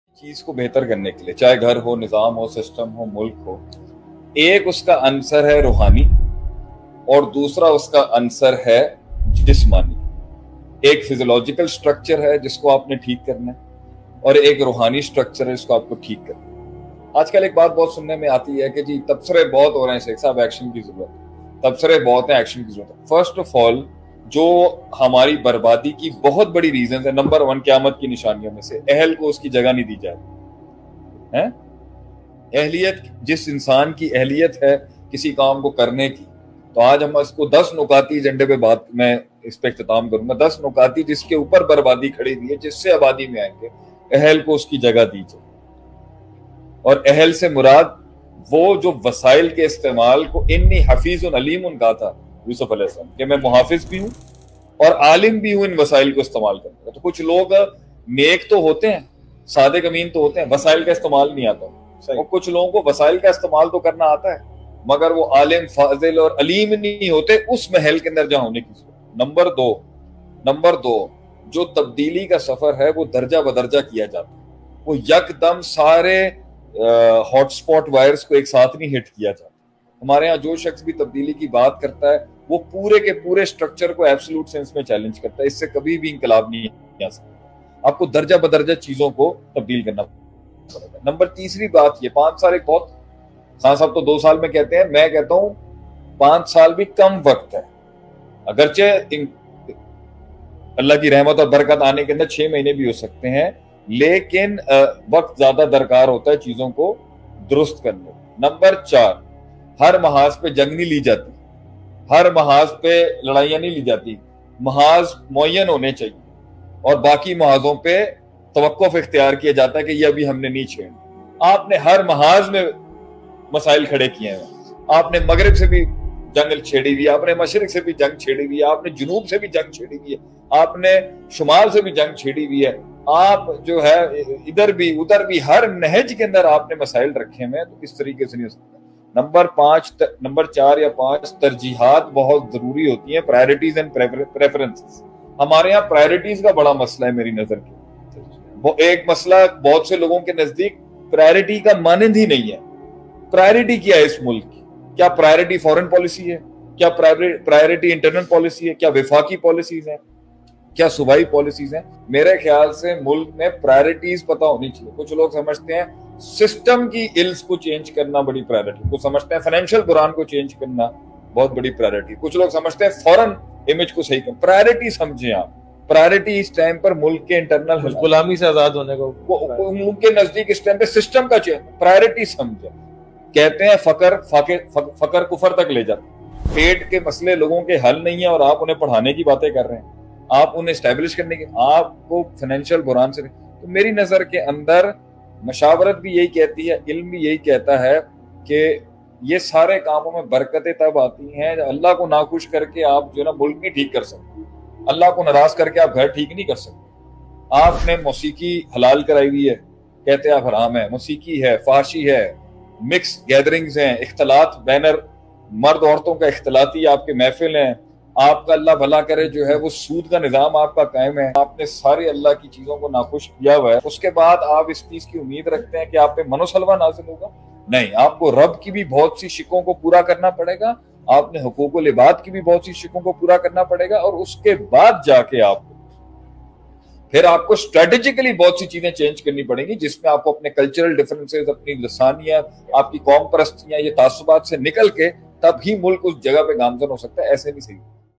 Very Important Speech: Hamari Barbadi kee Badi Wajuhat - Shaykh Atif Ahmed 2023
In this powerful and thought-provoking speech, sheikh atif ahmed delivered a powerful bayan on the theme of "Hamari Barbadi kee Badi Wajuhat"

Video and Audio Credit: @ShaykhAtifAhmedOfficial & @AlMidrarInstitute